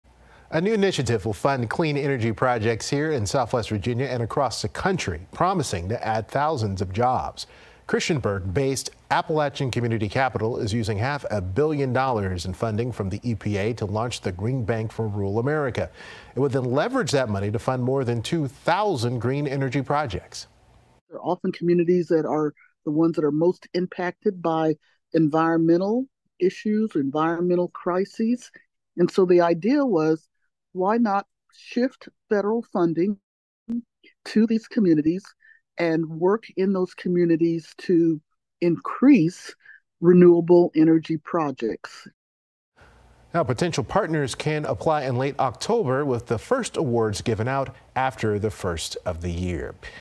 New initiative funding clean energy in Southwest Virginia
A new initiative will fund clean energy projects here in Southwest Virginia and across the country promising to add thousands of jobs.